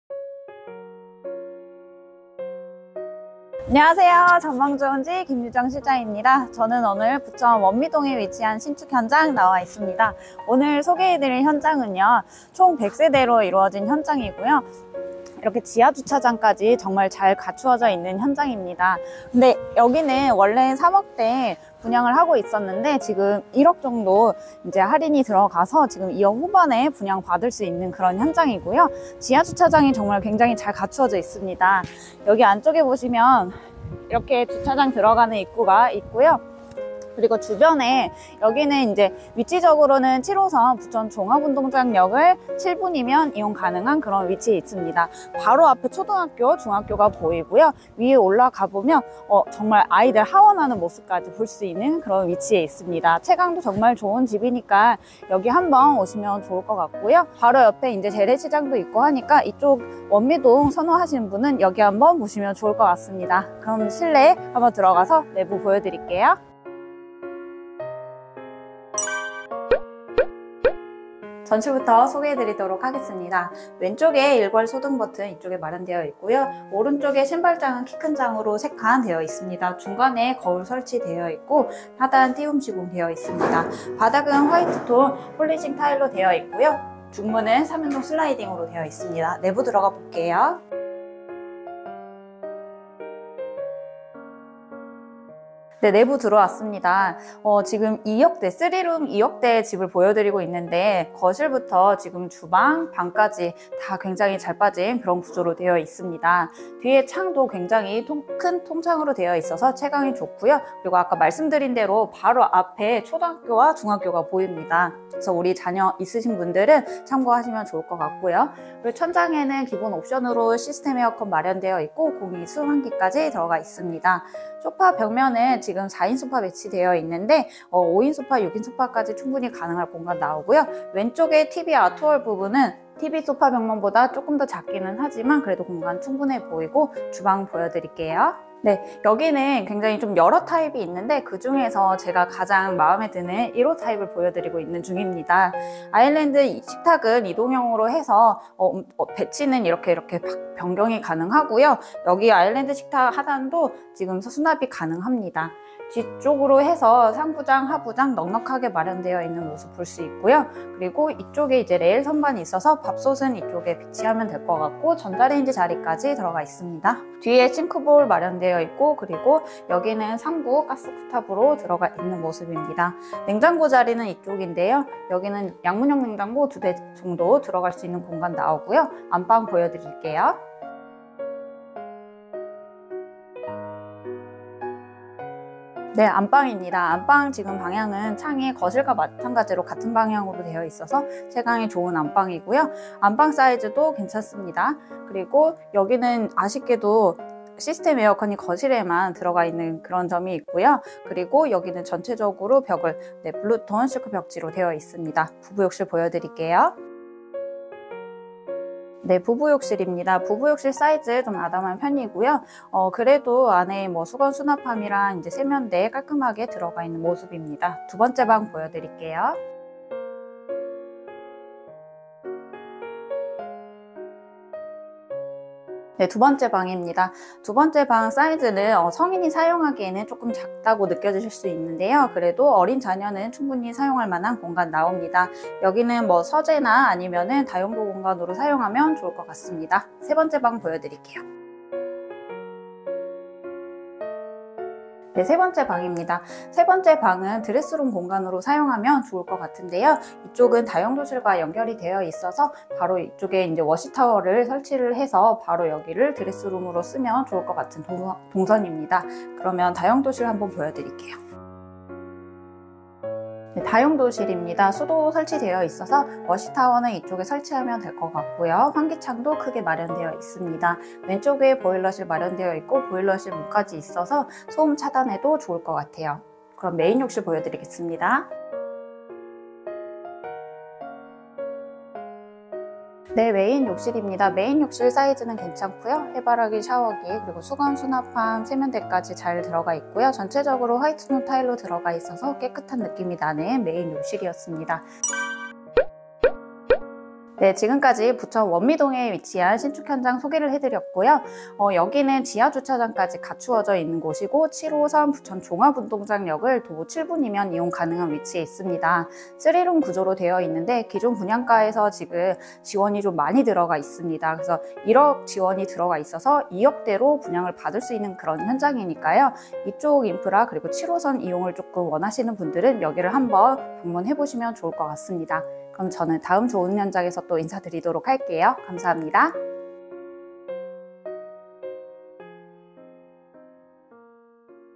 [부천신축아파트] 떳다!!!😲 마피 -1억 붙어서 2억대로 마련 가능한 원미동신축아파트｜7호선 부천종합운동장역에 GTX 개통되면 걍 돈벌듯 (No.1483) [부천신축빌라]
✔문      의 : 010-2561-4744

✔위      치 : 부천시 원미동
✔구      조 : 27평형 / 방3개 욕실2 베란다1
✔특      징 : ★ 전세대 1억 지원이벤트 / ★ 3룸 2억대로 분양 가능한 현장! / 거실 막힘없는 조망 / 바닥 전체 고급 강화마루 시공 /  주차 130% (지하주차장 있음)

✔옵      션 : 시스템에어컨, 공기순환기, 3구 가스레인지, 아일랜드 식탁
✔편의시설 : 7호선 부천종합운동장역 이용 / 부일초, 원미중 도보 3분 / 시민소체육공원 도보 3분 / 원미종합시장 도보 5분

(은행 대출 감정으로 기존 계약자분들이나 입주한 세대에 영향을 끼칠 수 있어 현장 요청에 의해 온라인상 금액 노출이 어려운 점 양 해 부탁드립니다)